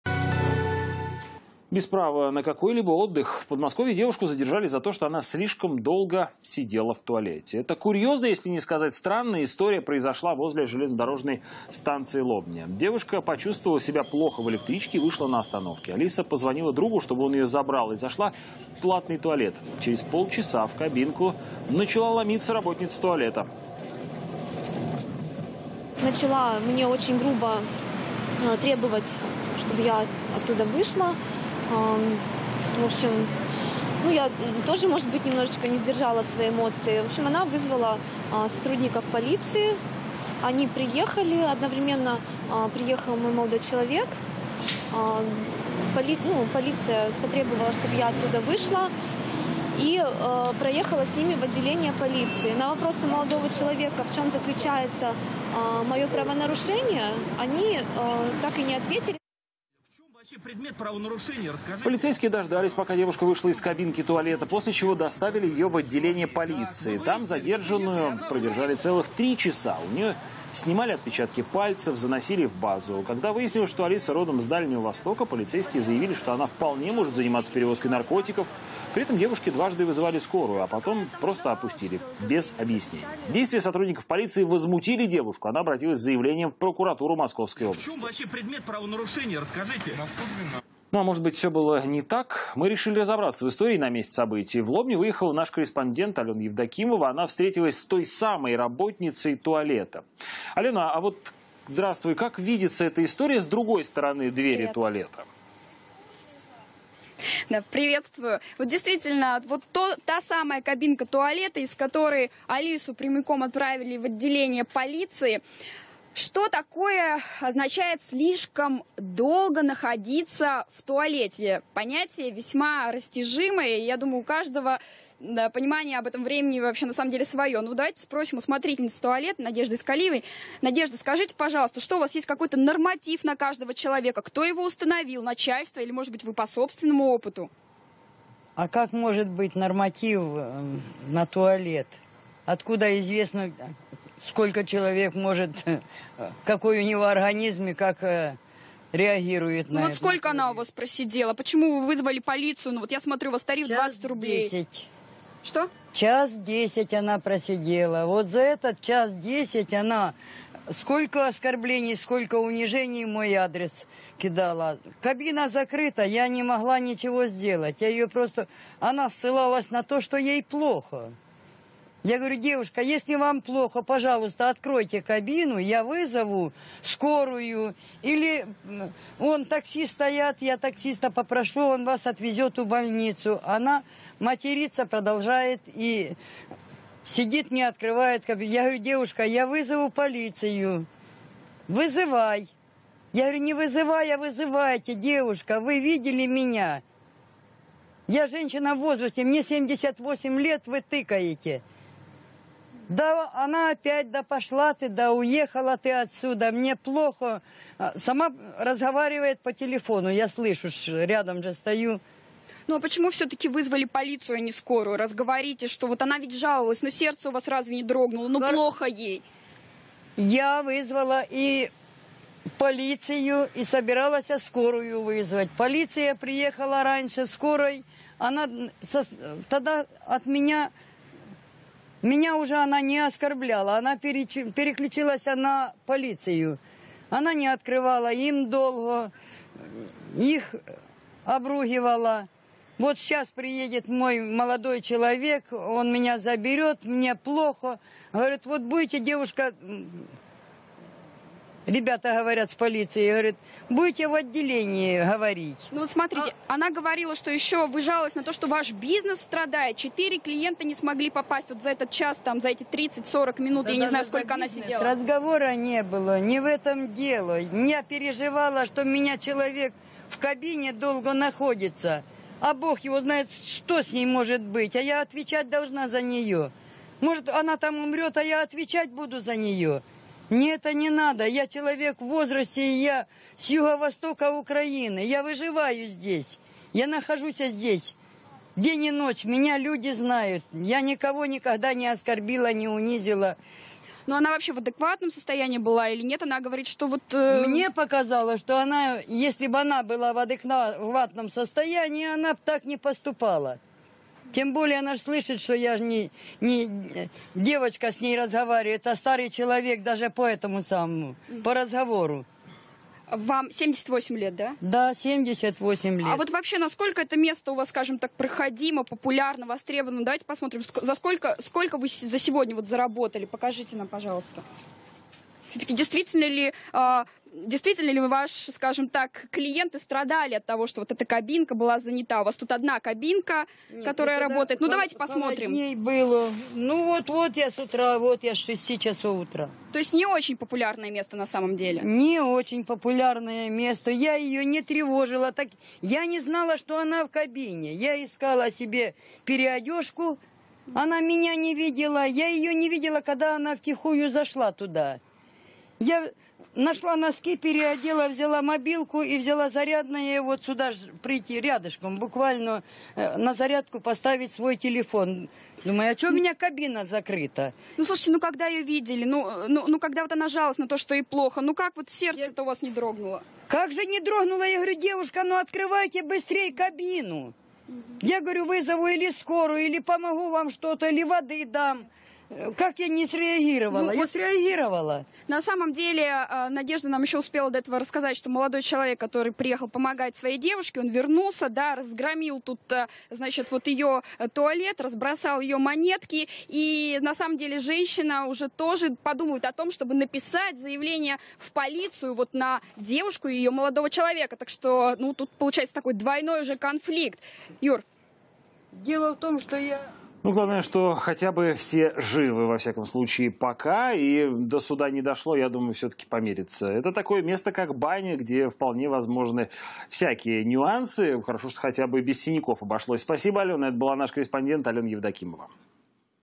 Полиция в Лобне задержала больную девушку за долгое сидение в туалете
Пострадавшая без объяснений причин продержали в отделении 3 часа